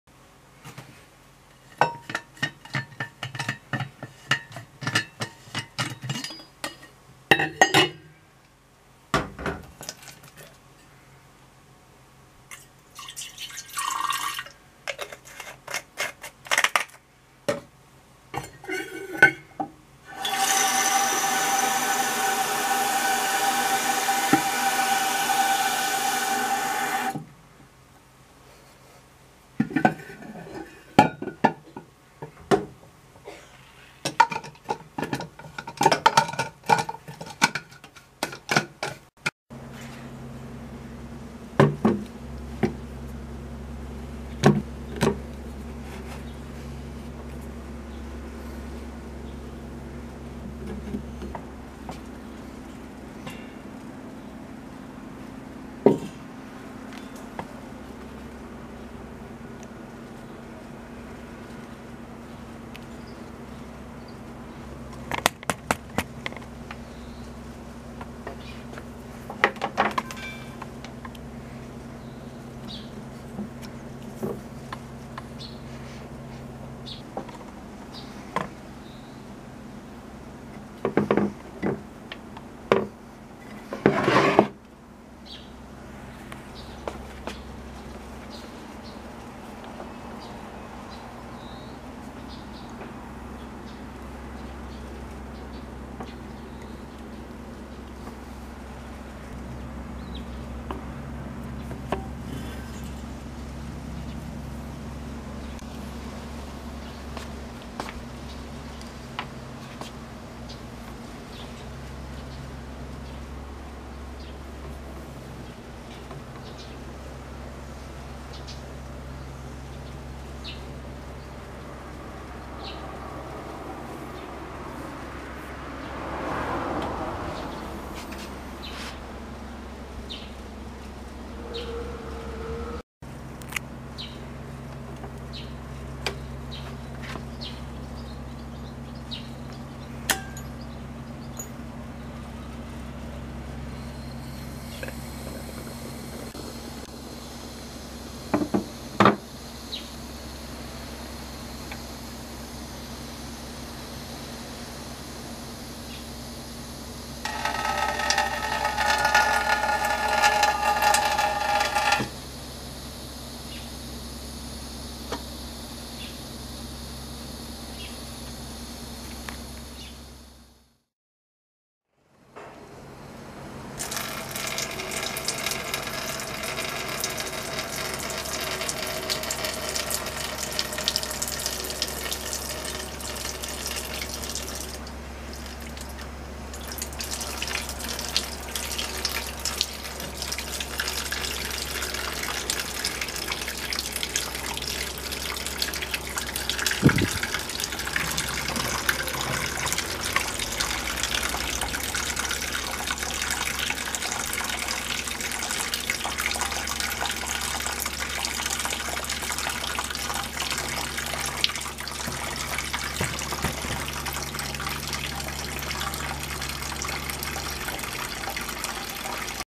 122 ビールサーバー洗浄 「おいしくビールを飲むために」How to clean beer server.
個人で購入したビールサーバー。ビールを飲んだ後は、ビールの配管に水を流して残っているビールを洗い流しています。配管をブラッシングすることはできず、なので、洗浄液を使って配管をきれいにしています。すべてはおいしくビールを飲むために。